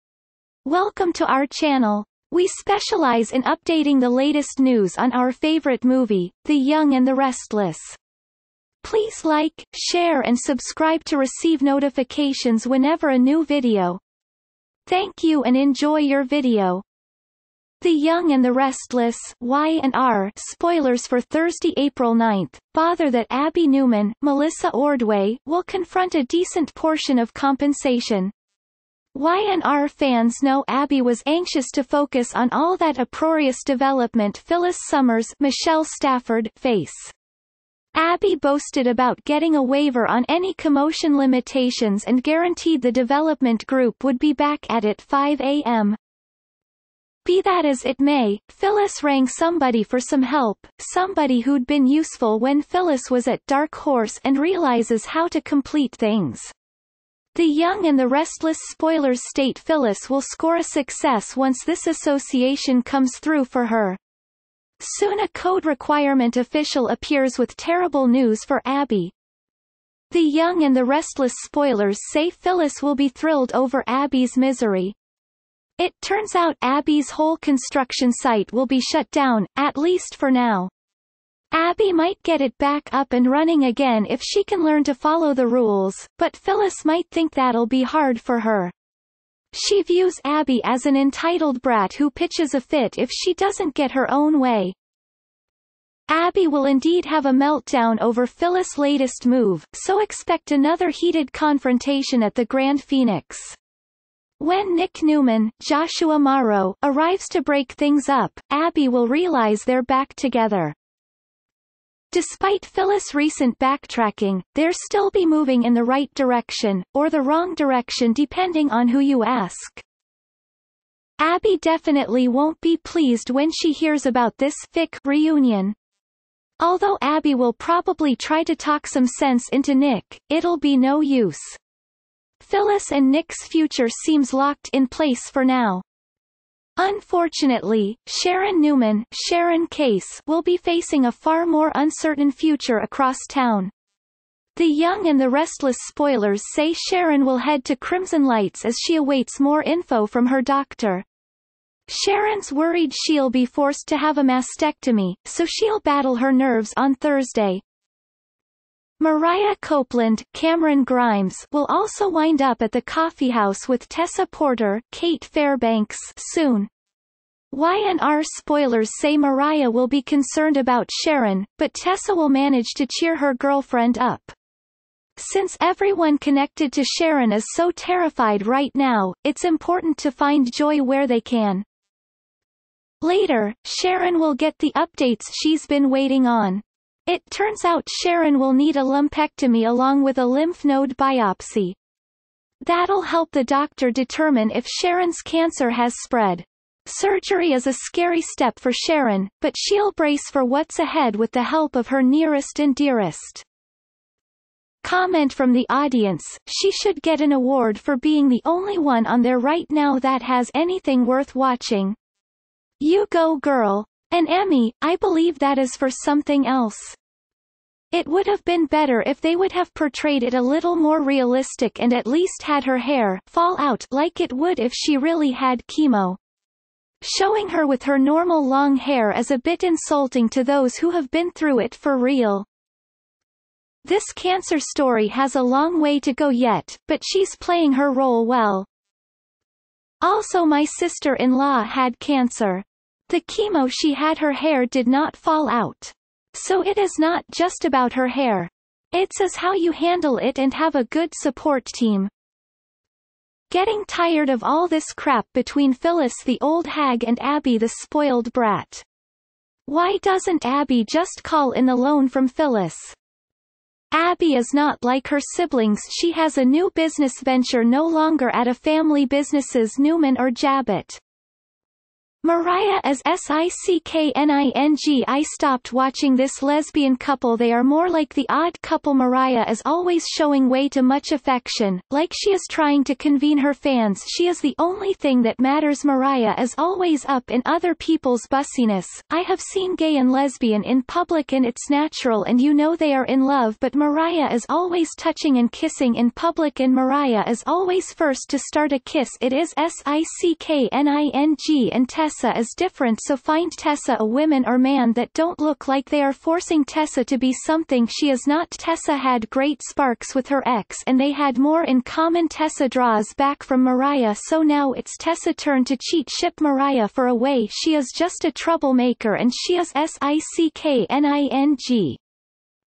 Y&R Update 04/09/20 | Phyllis thrilled over Abby’s misery - Sharon's Next Scary Step
Y&R Update 04/09/20
Phyllis thrilled over Abby’s misery - Sharon's Next Scary Step